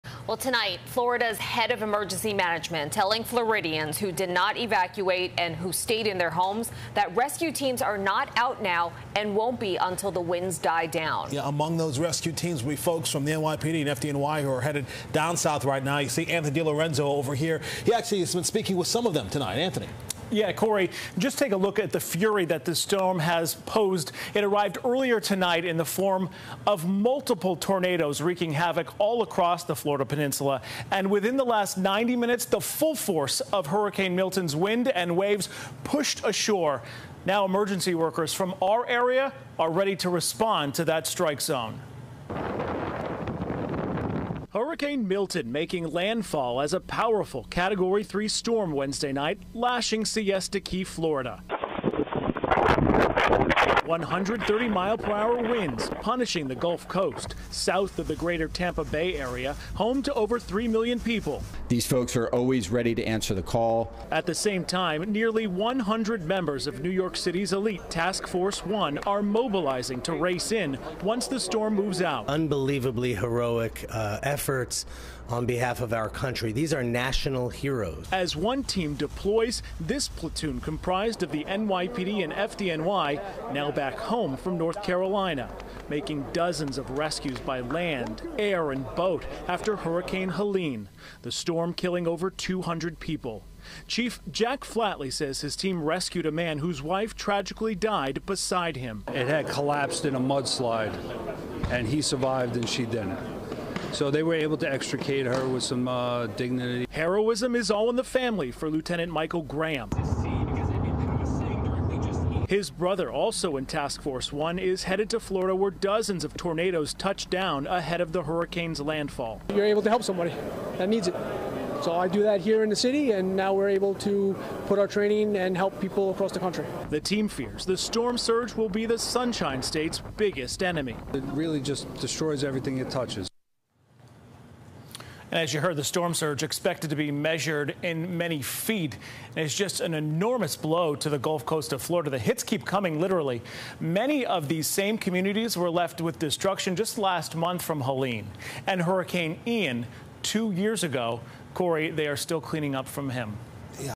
Hurricane Milton makes landfall in Florida; NYC heroes to help with search and rescue
Hurricane Milton made landfall Wednesday along Florida’s Gulf Coast as a Category 3 storm, bringing powerful winds, deadly storm surge and potential flooding to much of the state.